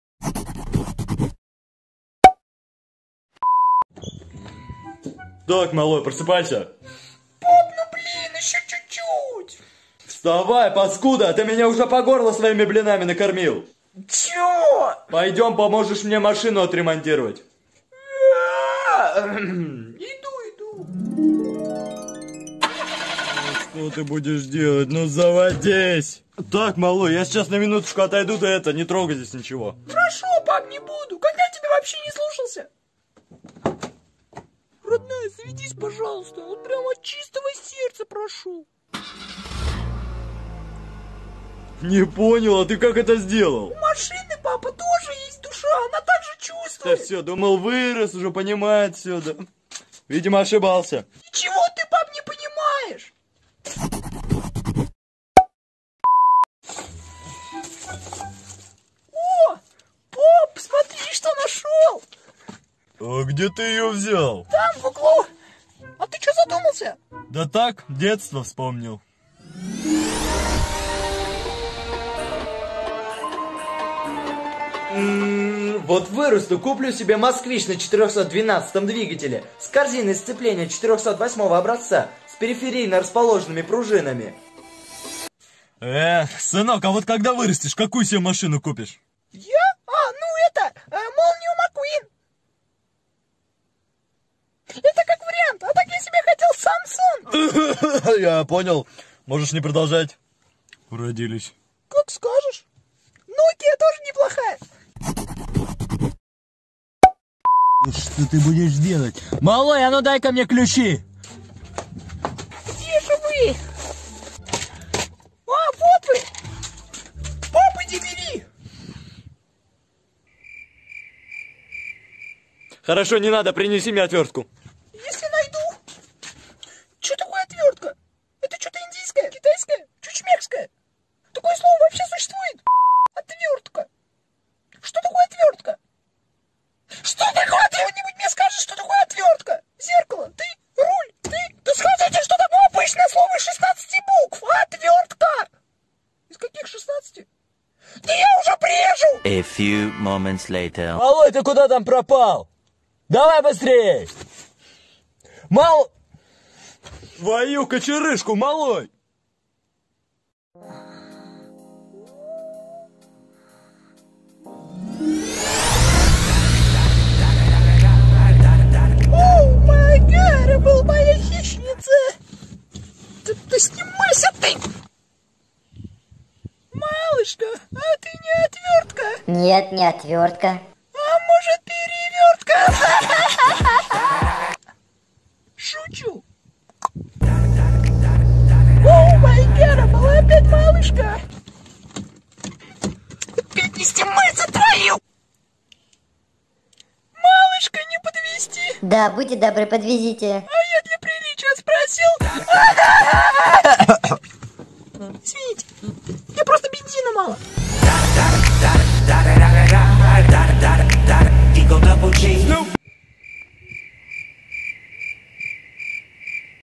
Сериал: Алик и его странная семейка. "Я У ПАПЫ В ГАРАЖЕ" - СЕРИЯ 2
Ребятки, а это вторая часть сериала: Алик и его странная семейка (Я У ПАПЫ В ГАРАЖЕ), где Алик пытался всячески помочь папе отремонтировать машину, но, как мы с Вами уже знаем, Алика всегда ждут какие-то приключения, ИНТЕРЕСНО?
Больше видео в моих Соц.Сетях:
 ▶️ПРОШЛОЕ ВИДЕО (БУДУЩЕЕ УЖЕ НАСТУПИЛО! ШОКИРУЮЩИЕ ОТКРЫТИЯ 21 ВЕКА С / ALIKONDRA)